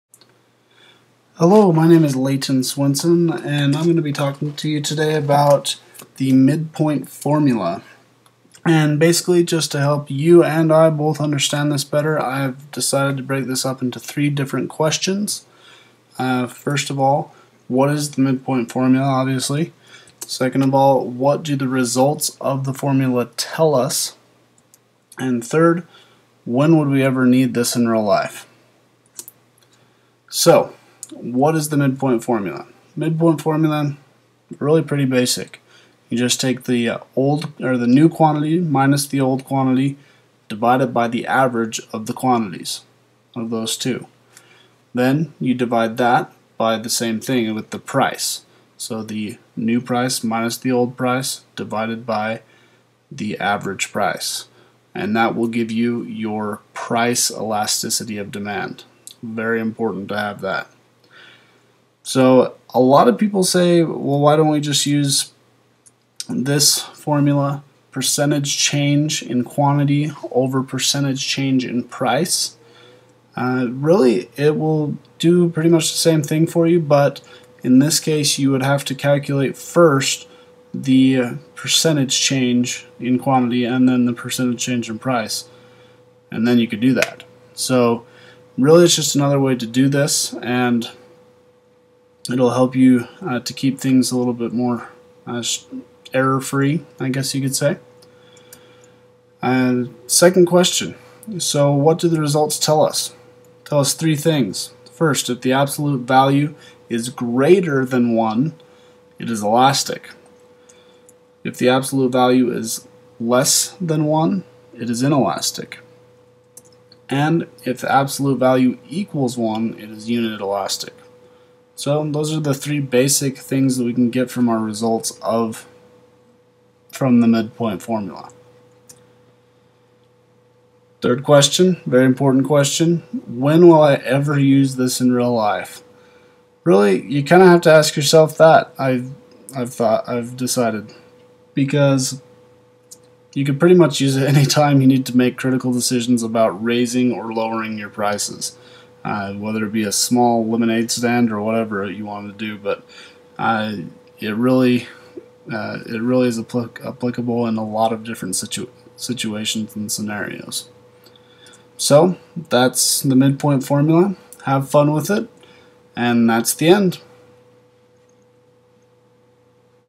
The Midpoint Formula elasticity Econ 150
How to calculate the price elasticity of demand with the midpoint formula.

Here is a link to an affordable book on basic economics for you to check out.